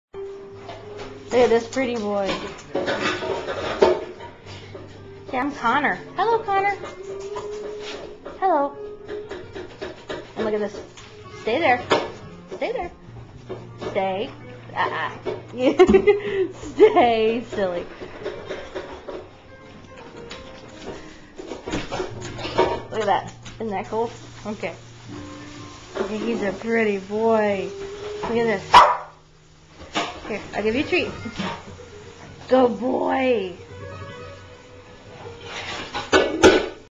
Rhodesian Ridgebacks are cool
This video is of my brother-in-law's ridgeback, Connor. Connor is a really cool animal, and very sweet and graceful. I took this to show off his ridge. Recorded on December 25, 2008 using a Flip Video camcorder.